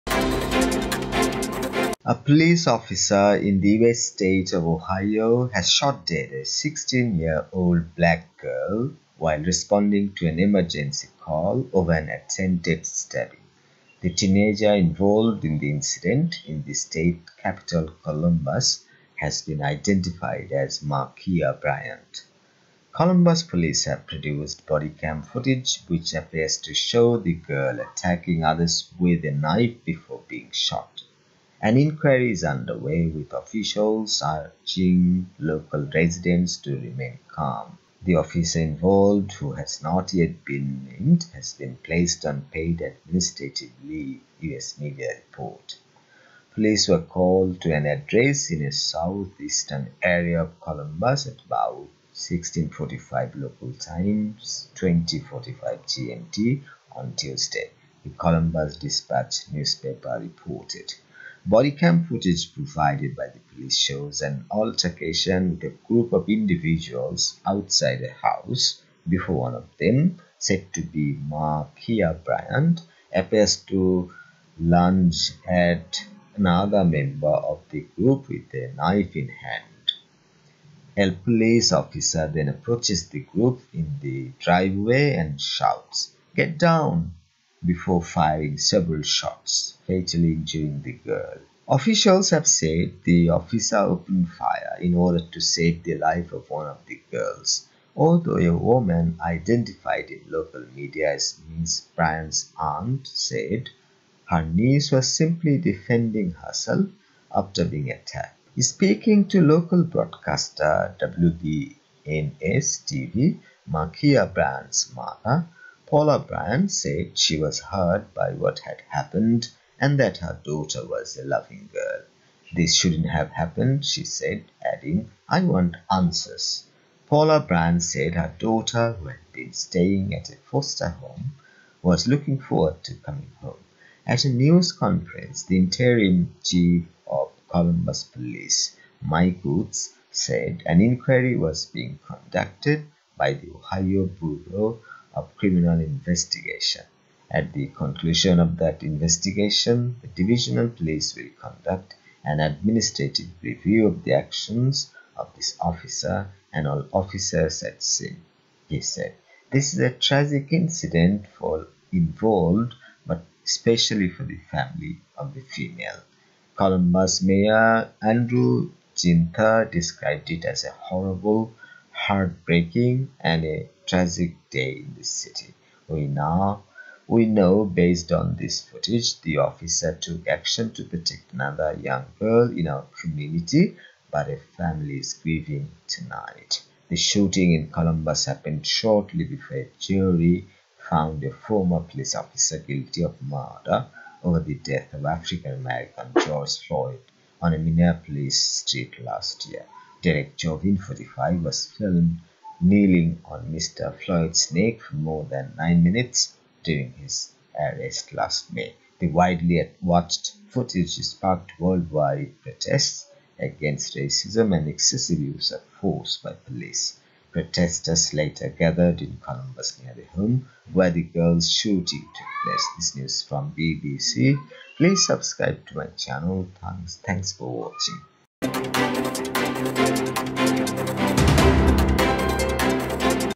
Black teenage girl shot dead by police in Ohio
A police officer in the US state of Ohio has shot dead a 16-year-old black girl while responding to an emergency call over an attempted stabbing.

The teenager involved in the incident, in the state capital Columbus, has been identified as Ma'Khia Bryant.

Columbus police have produced bodycam footage which appears to show the girl attacking others with a knife before being shot.

An inquiry is under way, with officials urging local residents to remain calm.

The officer involved, who has not yet been named, has been placed on paid administrative leave, US media report.

Police were called to an address in a south-eastern area of Columbus at about 16:45 local time (20:45 GMT) on Tuesday, the Columbus Dispatch newspaper reported.

Bodycam footage provided by the police shows an altercation with a group of individuals outside a house before one of them - said to be Ma'Khia Bryant - appears to lunge at another member of the group with a knife in hand.

A police officer then approaches the group in the driveway and shouts "get down" before firing several shots, fatally injuring the girl.